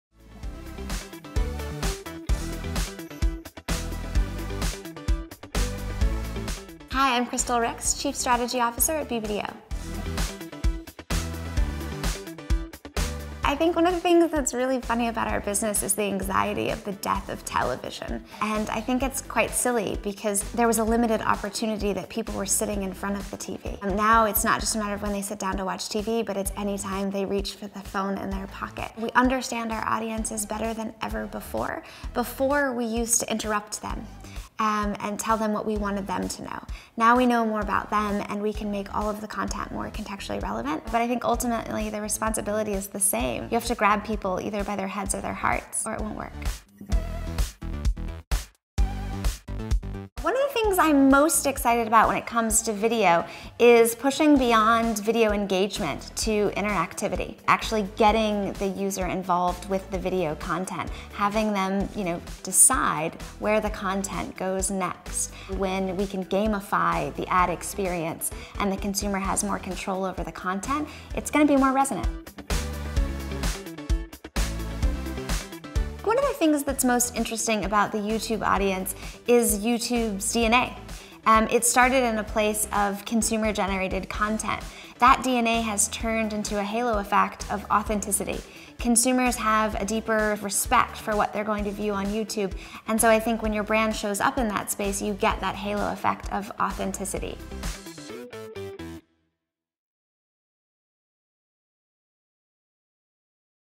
This is My YouTube: Crystal Rix, Chief Strategy Officer at BBDO NY | YouTube Advertisers
We asked some of advertising’s most influential leaders what everyone should know about online video. Hear how Crystal Rix thinks about YouTube professionally, and how she uses it personally.